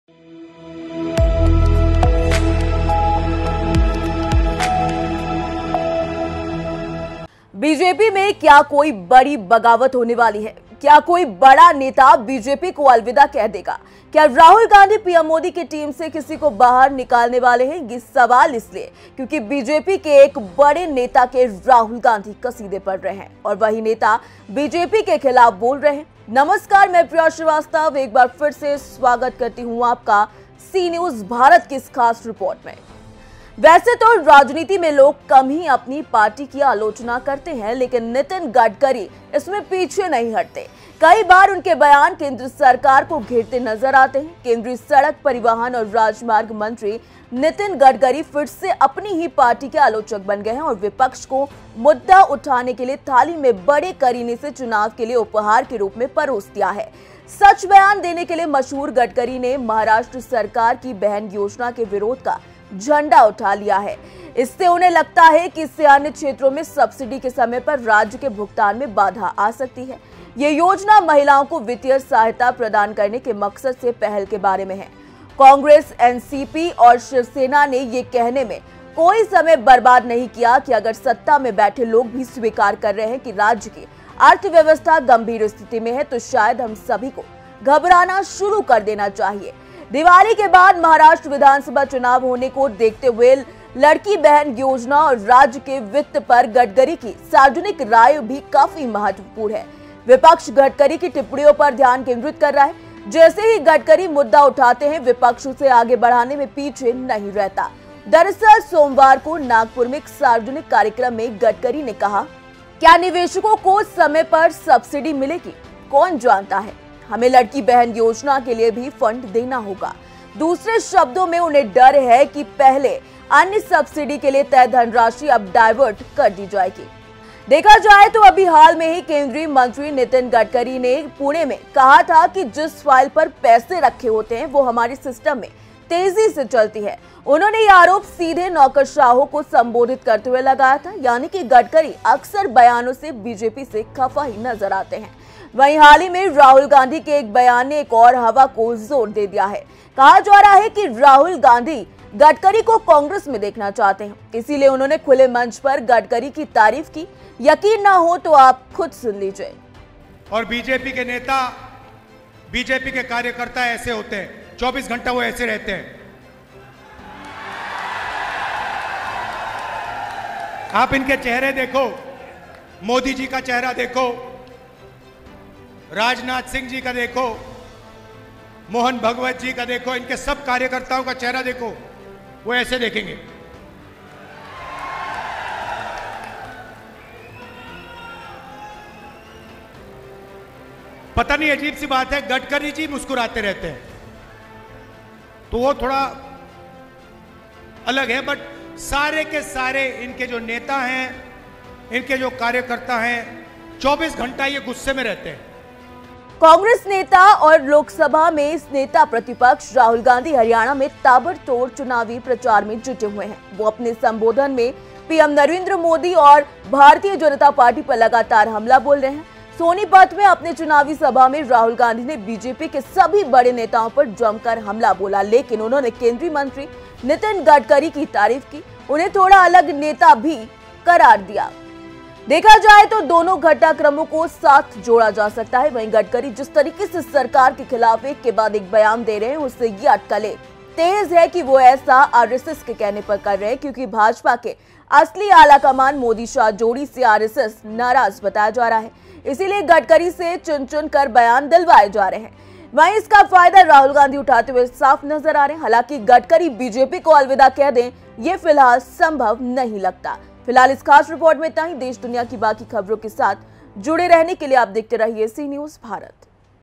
नितिन गडकरी ने अपनी ही सरकार को घेरा, राहुल से मिलाएंगे हाथ || Cnews Bharat
नितिन गडकरी अपनी ही सरकार पर कर रहे हैं हमले
चुनावी मंच पर राहुल गांधी ने की नितिन गडकरी की तारीफ
अटकलें तेज हैं कि गडकरी आरएसएस के कहने पर दे रहे बयान